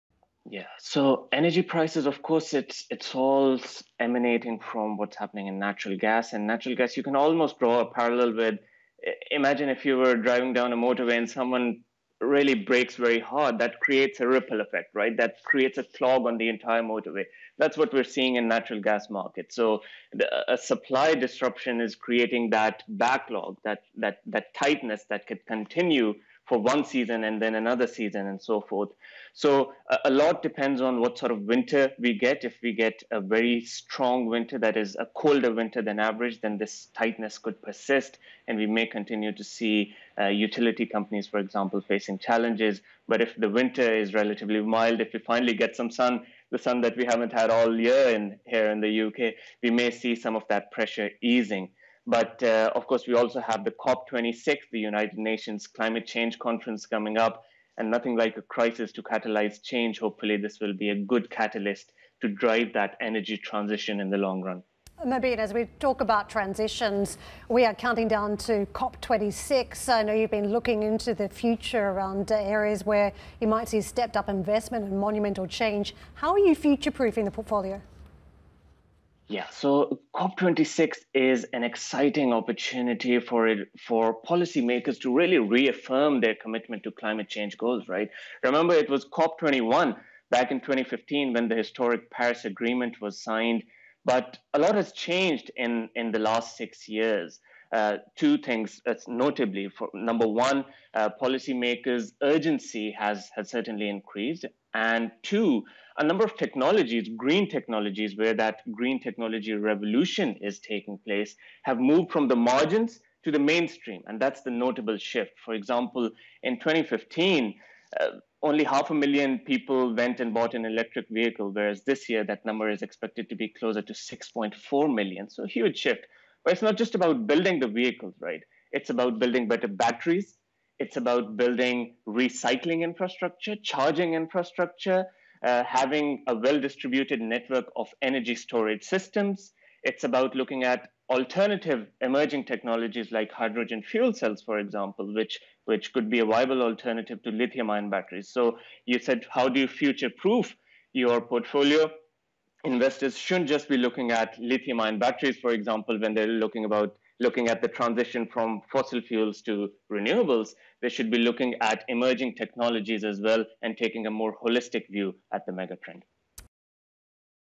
Investors need to take a more holistic view of the energy transition, researcher says
Mobeen Tahir, associate director of research WisdomTree, said investors shouldn't just be looking at investing in lithium-ion batteries when it comes to the energy transition.